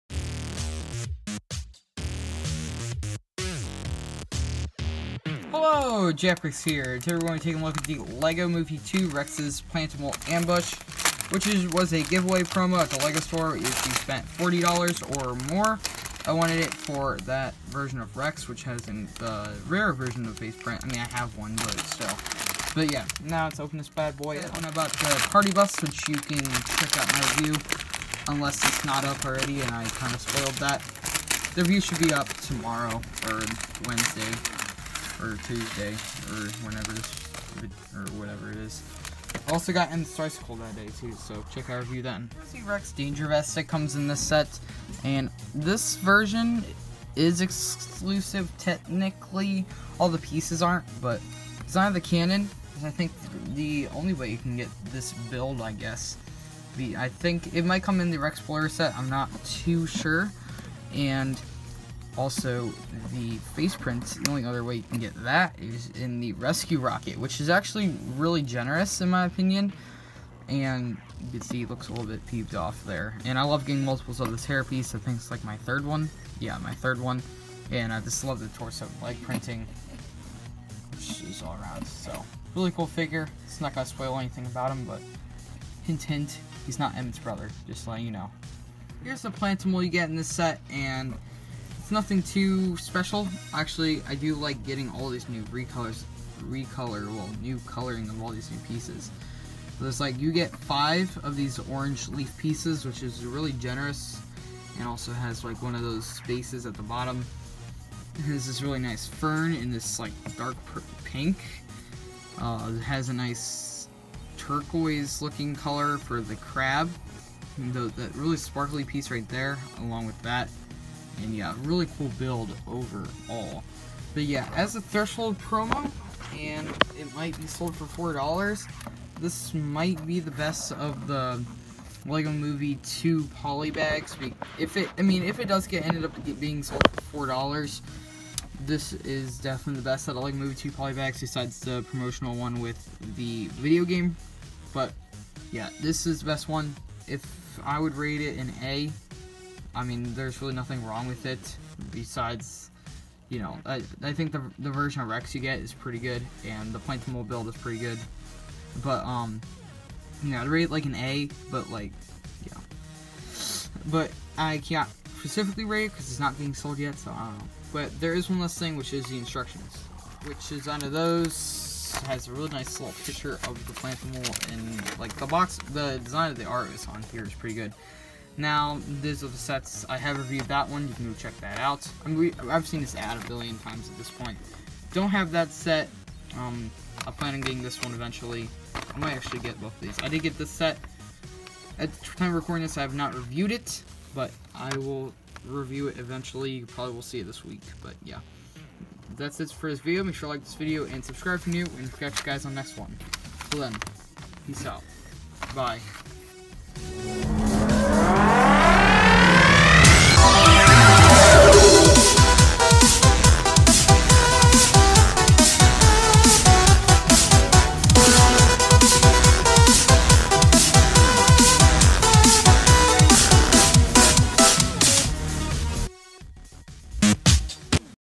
Rare LEGO Movie 2 Rex’s Plantimal Ambush Review! 2019 Polybag 40460!
I review The LEGO Movie 2 Rex’s Plantimal Ambush Polybag! It was a promotion I got a couple days ago!